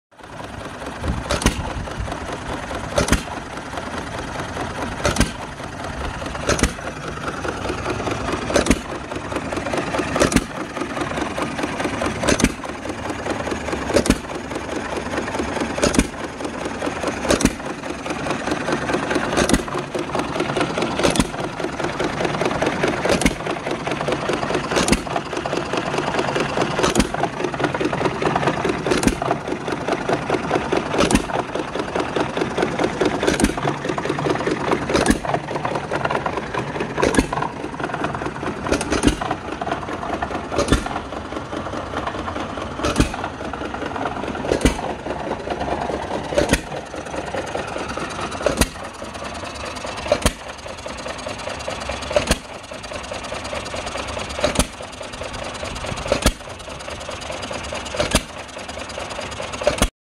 John Deere Hit 'N Miss Ice Cream Machine
John Deere hit n miss engine hooked up to an ice cream maker. Will make 5 gallons of ice cream at a time.